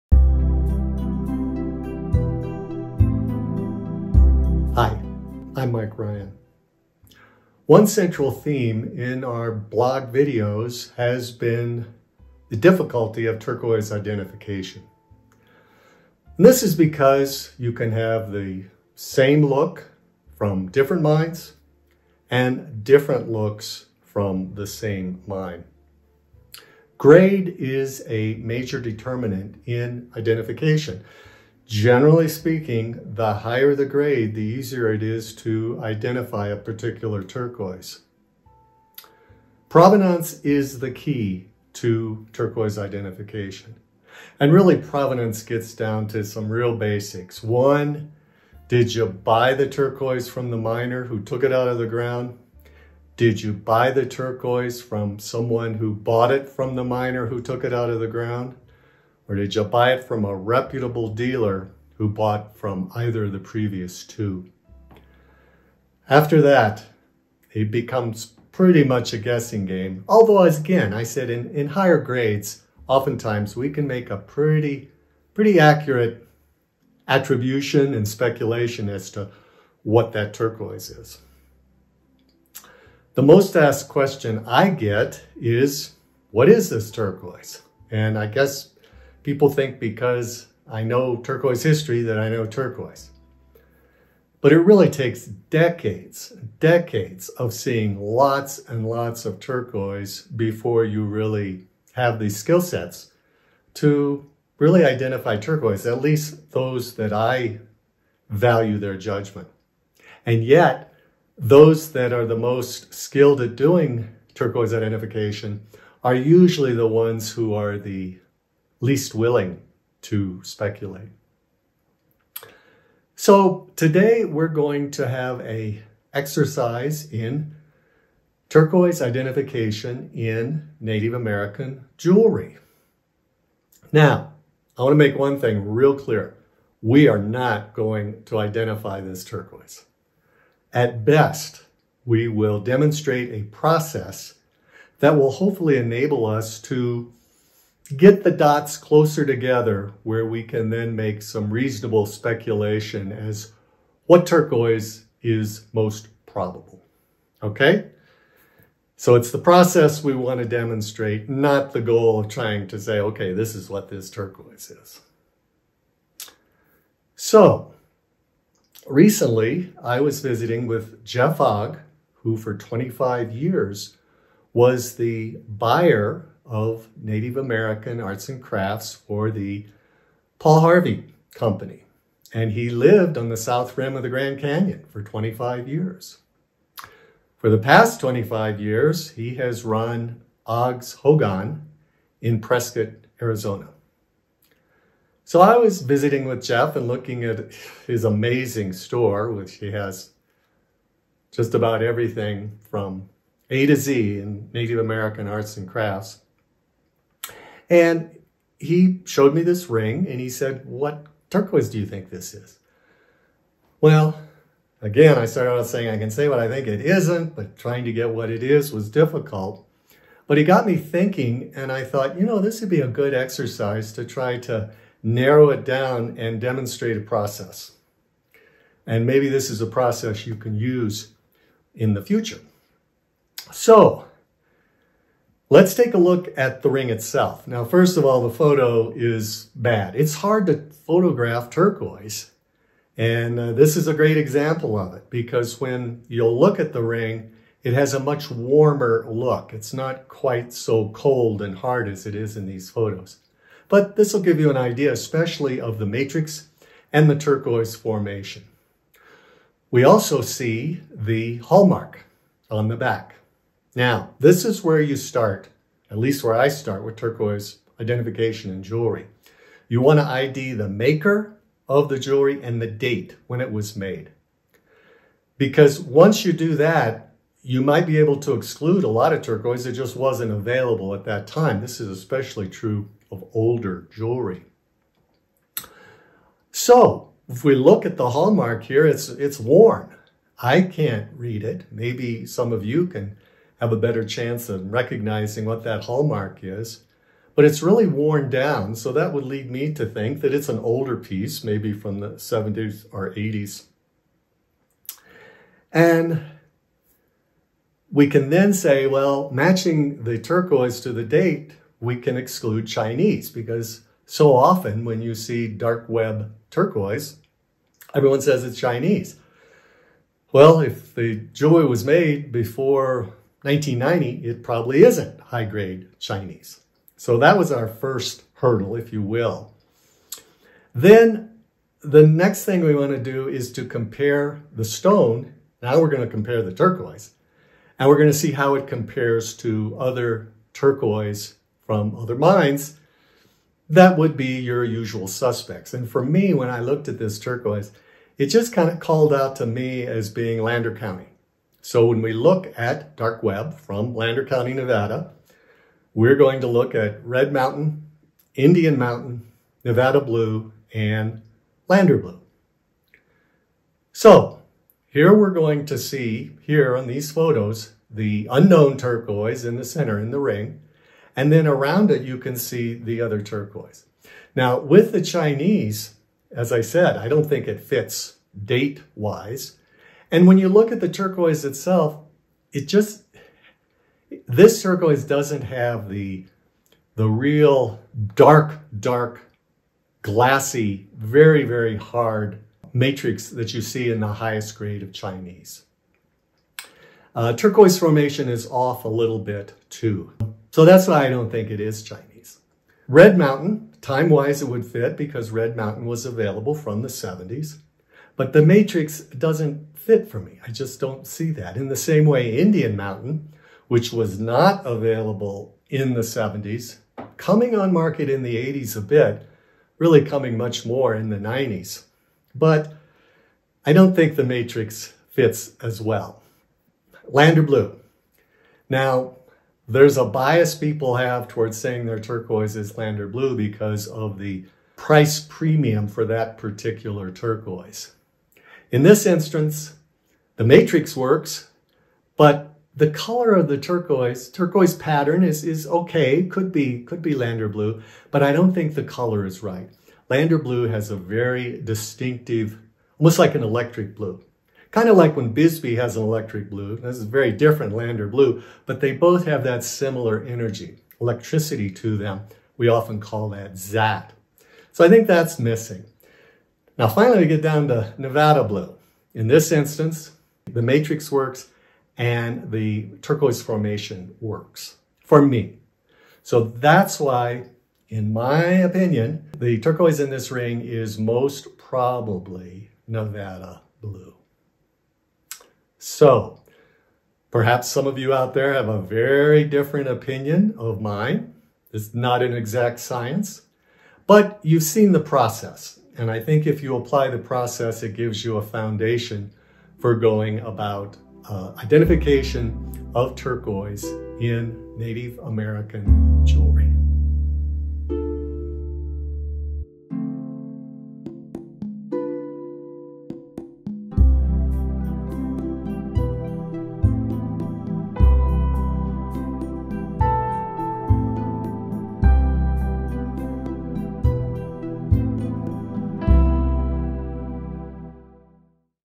Turquoise Identification in Jewelry
A frequent question is for identification of turquoise in Native American jewelry. Viewers know how difficult that may be. In this video Mike presents a step by step method for helping to identify turquoise in jewelry.